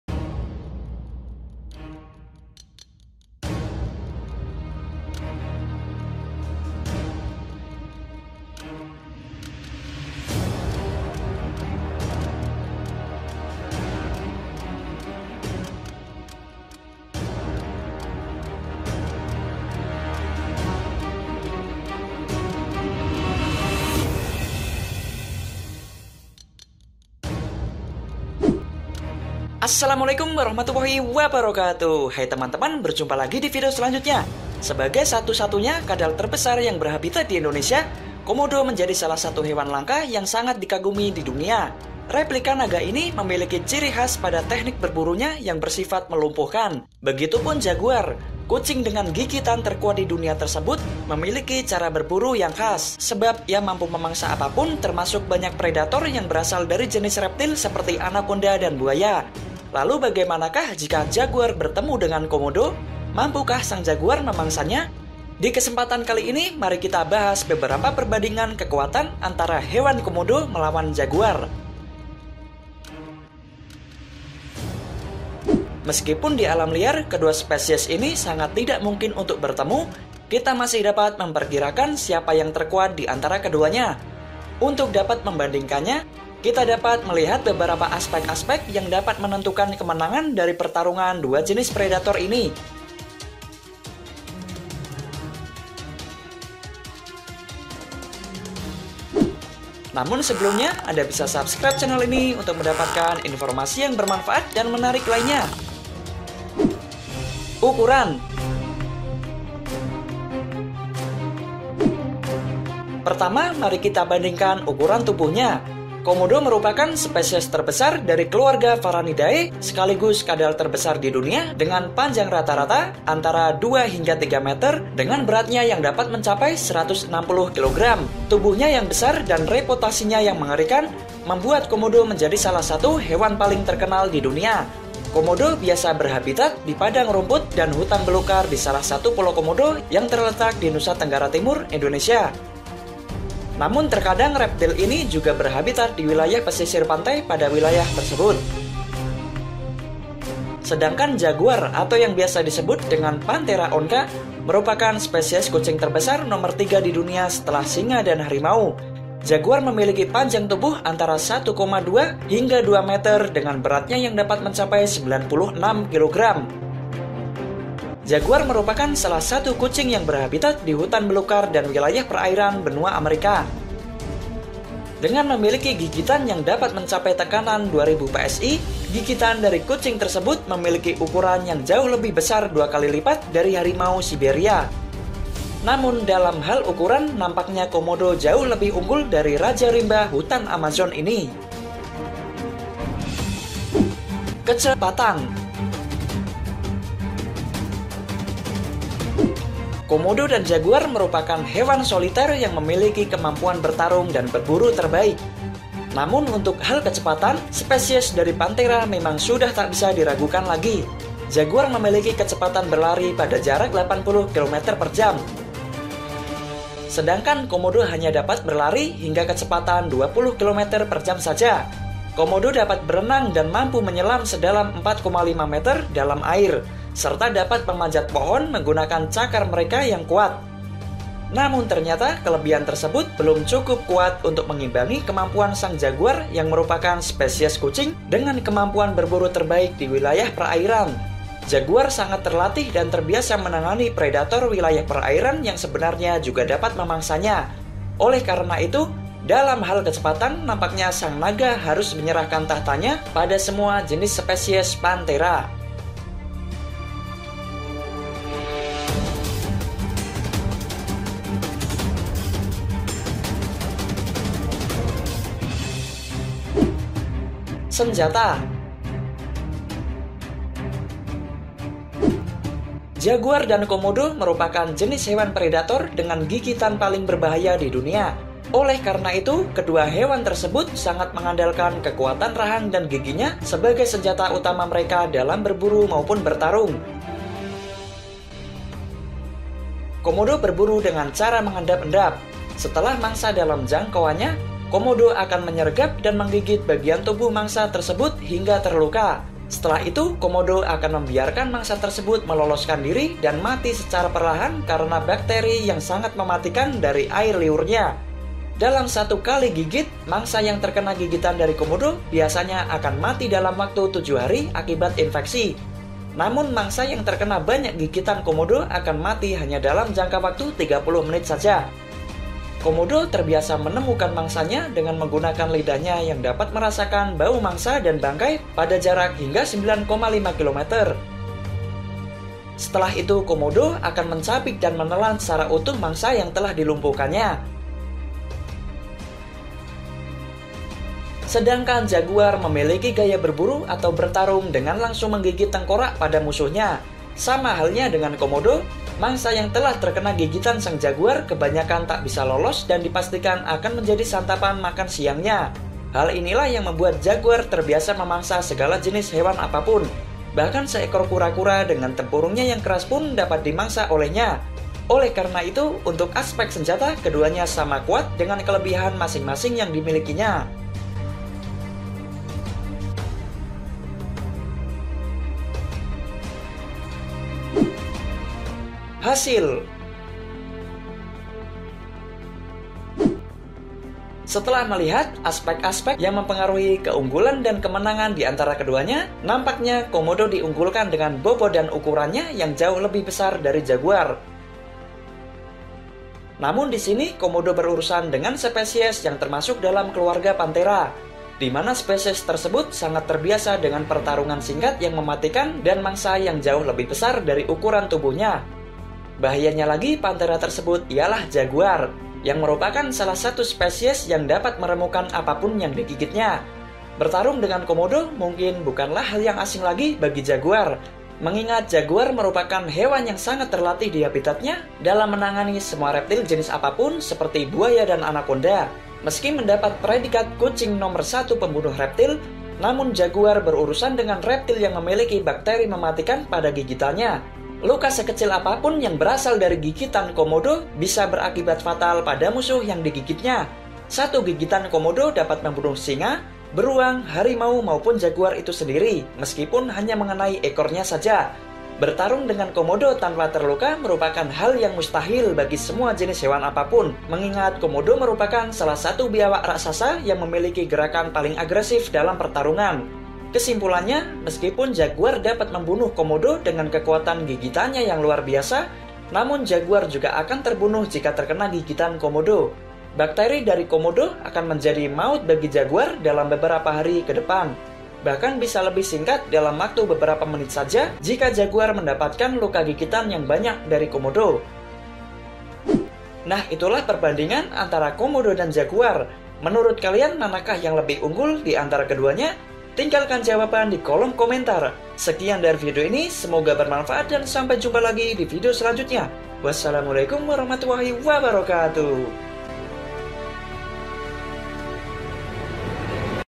Komodo VS Jaguar (Pertarungan Hewan dengan Gigitan Paling Mematikan di Dunia)
Komodo merupakan satu satu nya reptil kadal terbesar di dunia. Sebagai satu satu nya kadal terbesar yang berhabitat di indonesia, komodo menjadi salah satu hewan langka yang sangat di kagumi dunia. Replika naga ini memiliki ciri khas pada teknik berburu nya yang bersifat melumpuhkan. Begitupun jaguar.. kucing dengan gigitan terkuat di dunia tersebut memiliki cara berburu yang khas. Sebab ia mampu memangsa apapun termasuk banyak predator yang berasal dari jenis reptil seperti anaconda dan buaya. Lalu bagaimana kah jika jaguar bertemu dengan komodo..?? mampu kah sang jaguar memangsa nya?? Di kesempatan kali ini mari kita bahas beberapa perbandingan kekuatan antara hewan komodo melawan jaguar...